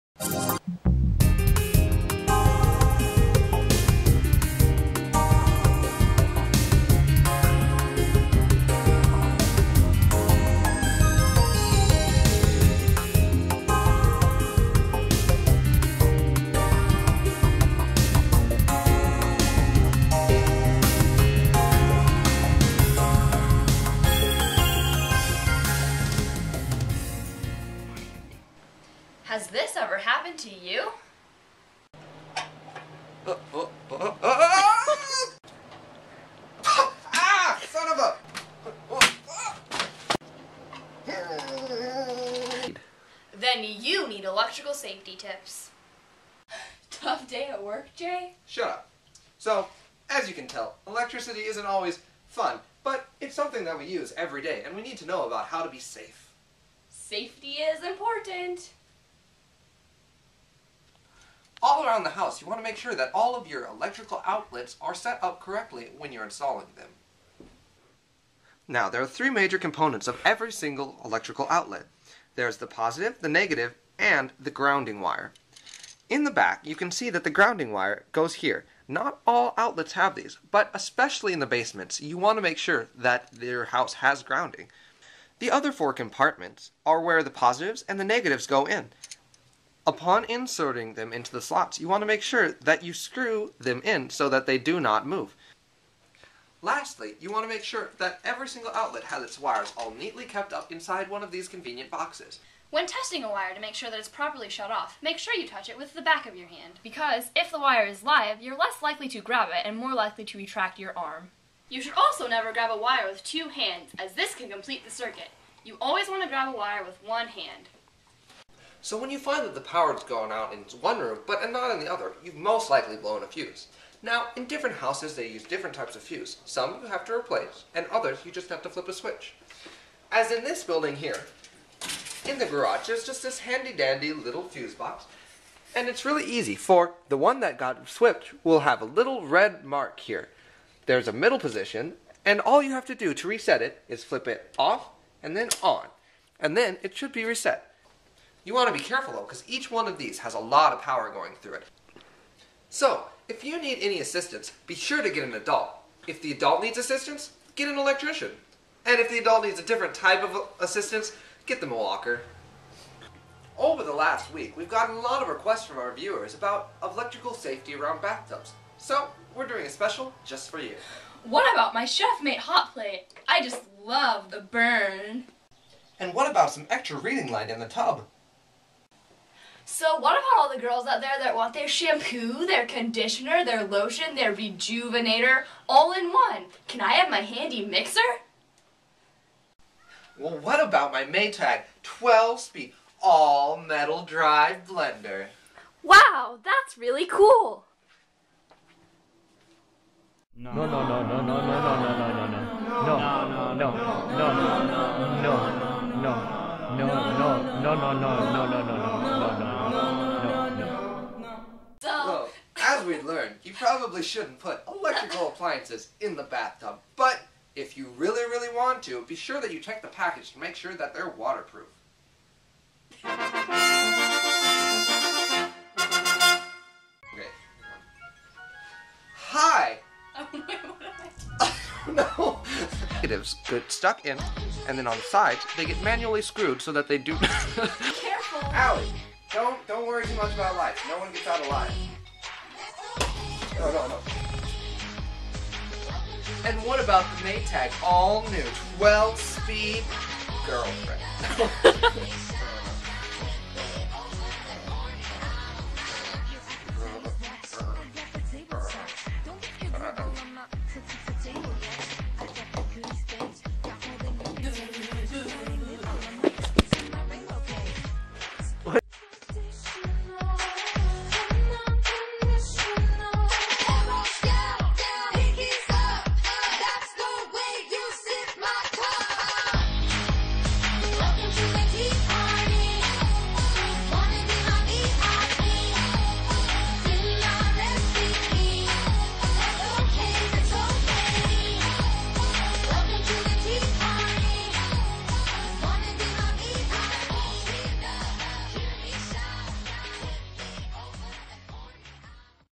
Electrical Safety VIDEO
Allie and Jay's project for physics. Spring 2010